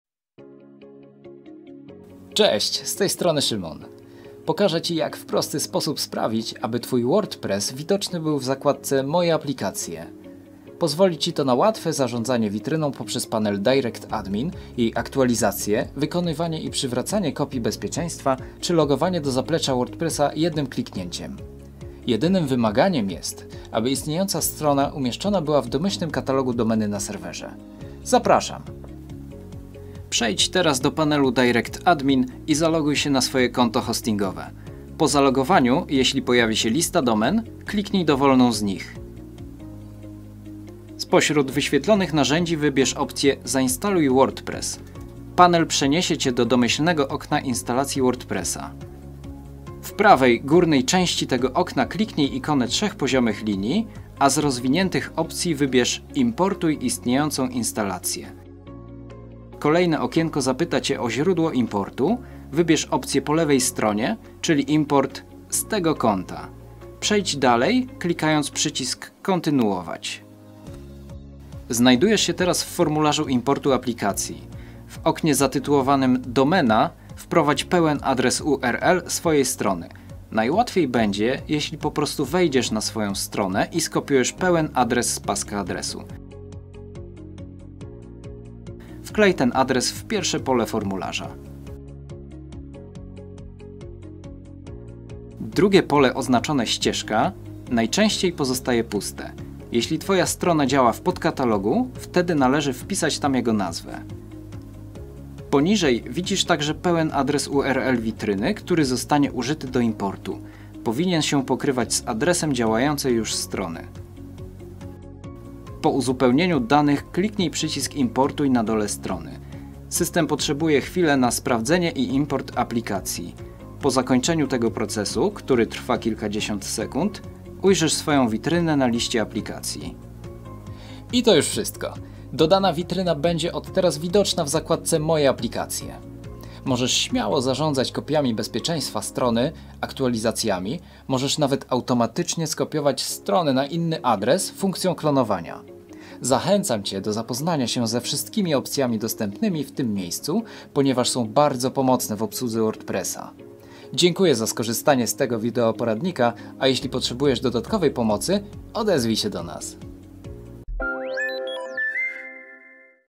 Jak ZAIMPORTOWAĆ istniejącą stronę WORDPRESS do DirectAdmin 👉 PORADNIK Krok po kroku
Jak zaimportować istniejącą instalację WordPress do directAdmin?
Zawartość filmu:
00:00 Wstęp
00:25 Jak zaimportować istniejącą instalację WordPress do directAdmin?
01:29 Jak podpiąć domenę do instalacji WordPress?
02:15 Zakładka Moje Aplikacje w cyber_Folks